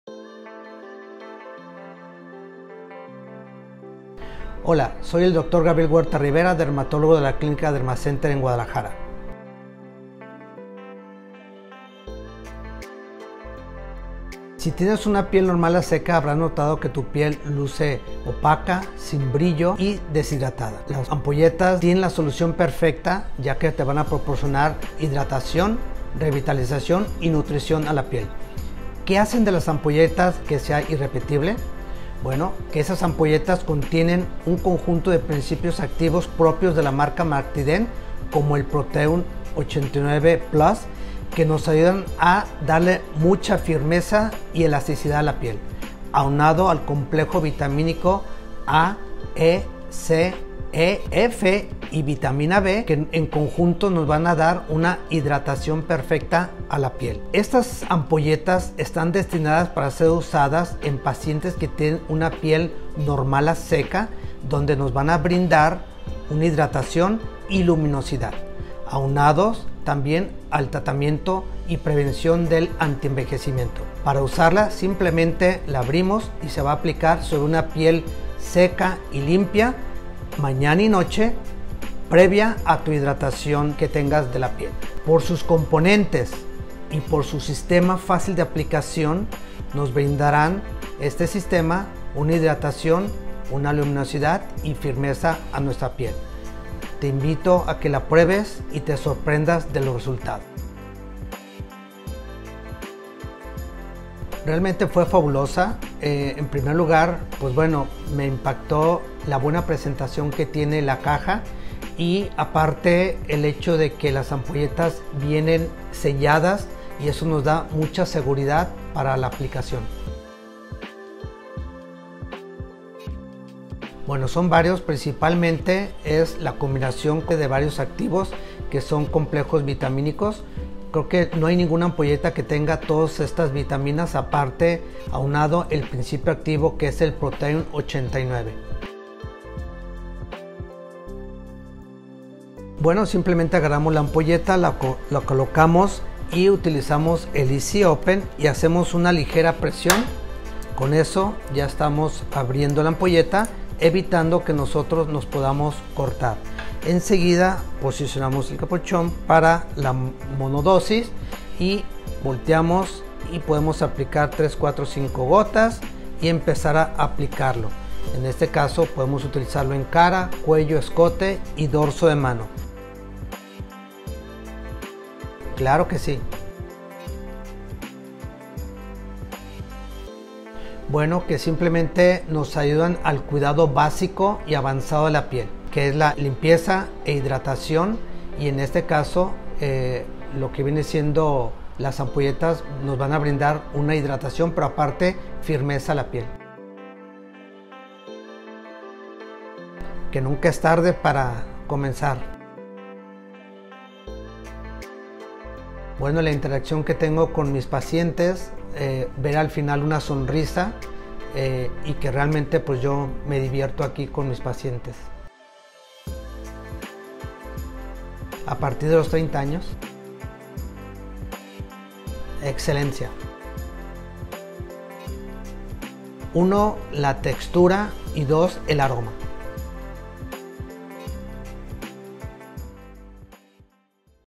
Ampolletas Skin Complex Advanced con Dr. Gabriel Huerta Rivera | MartiDerm México | DERMACenter
Las Ampolletas Skin Complex Advanced tienen una fórmula más completa para tratar el cronoenvejecimiento y/o de la piel desvitalizada. ✨

📍 Clínica DERMACenter Guadalajara

Aviso de Publicidad: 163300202D0036. CONSULTA A TU DERMATÓLOGO